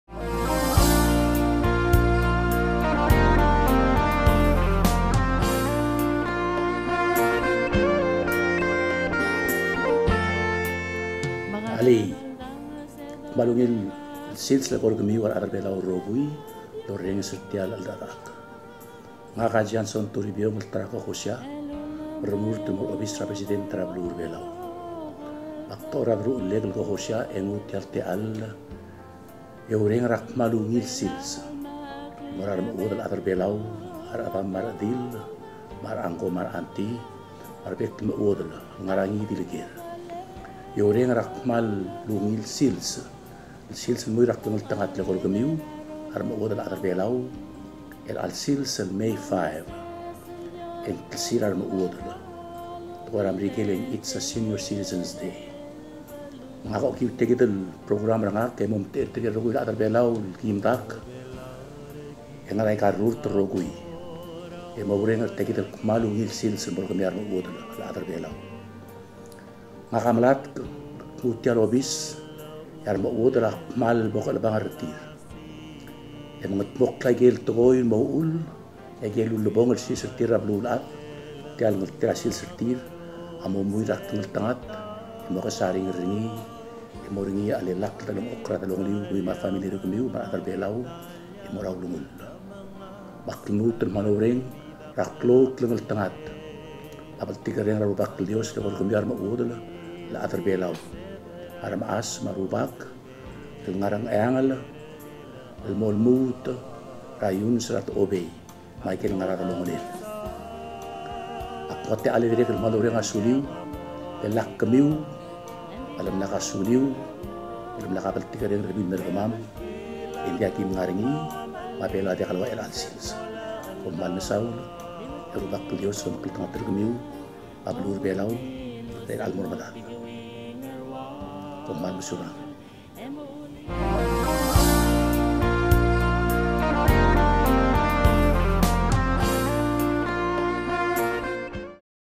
A message to all Senior Citizens in Palau and around the world from Johnson Toribiong himself.
A short message(in Palauan language) to Senior Citizens of Palau around the world by the 2020 Presidential Candidate of Palau, Johnson Toribiong.